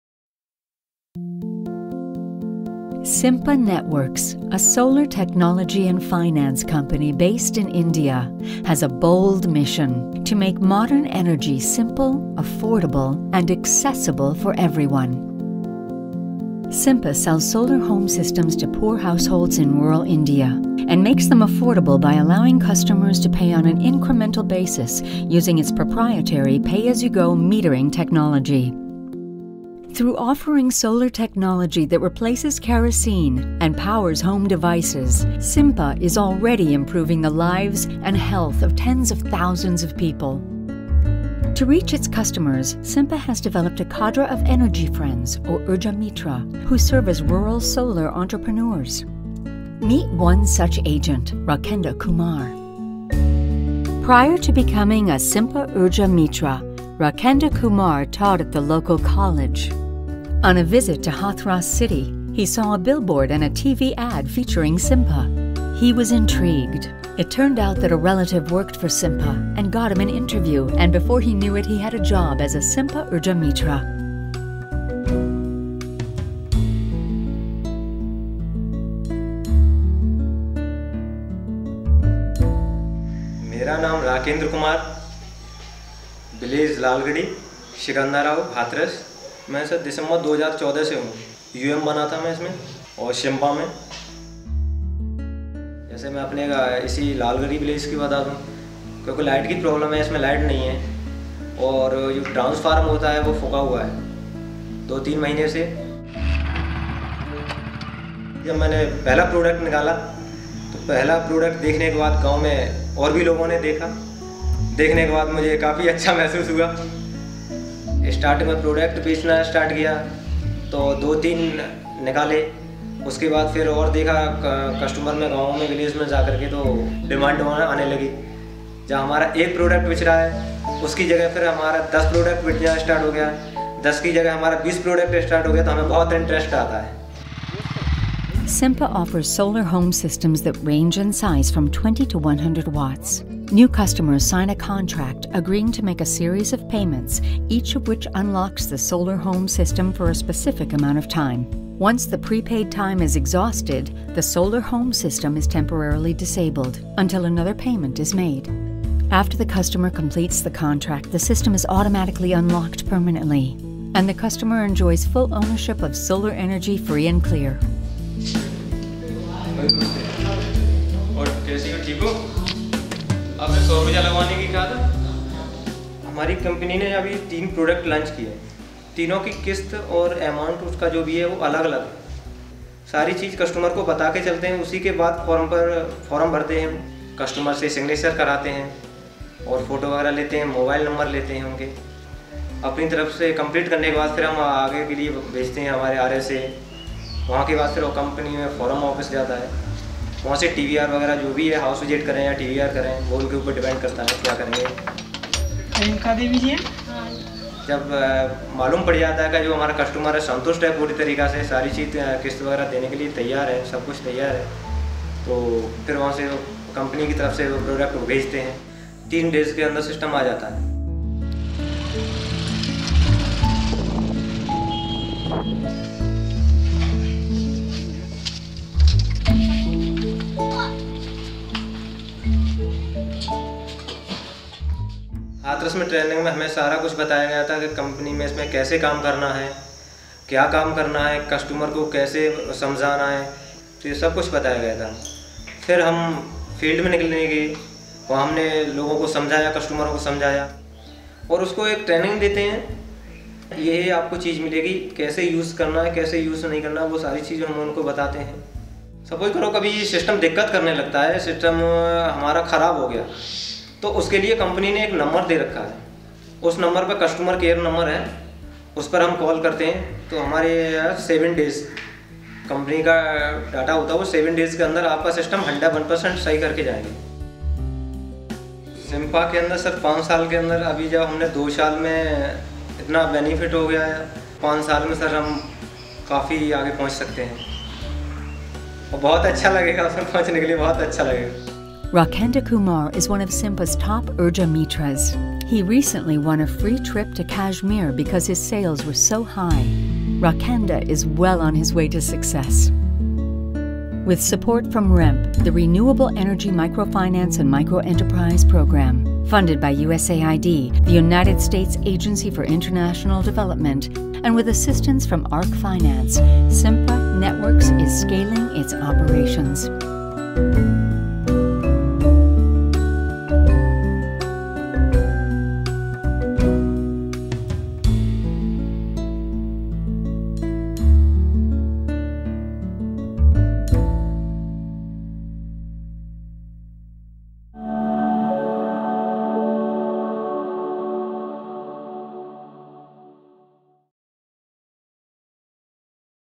Meet Rakendra: A Simpa Networks Solar Agent
Simpa Networks is a leading solar technology and finance company based in India that is selling solar home systems to poor households in rural India, and making them affordable by allowing customers to pay on an incremental basis using its proprietary “pay-as-you-go” metering technology. This video profiles the work of a Simpa Solar Agent as he helps clients gain access to reliable, modern energy.

With Arc’s technical assistance, Simpa is scaling its "progressive purchase" business model.